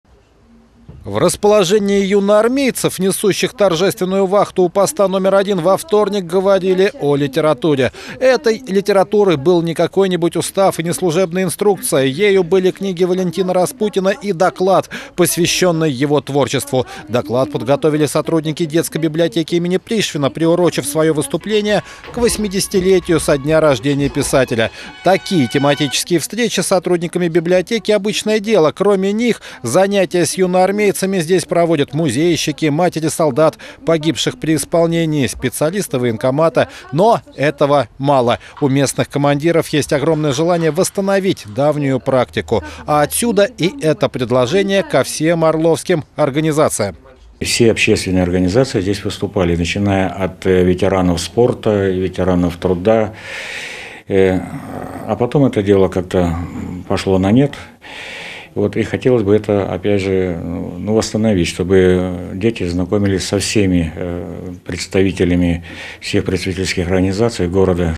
Литература на посту
В расположении юнармейцев, несущих торжественную вахту у Поста № 1, во вторник говорили о литературе.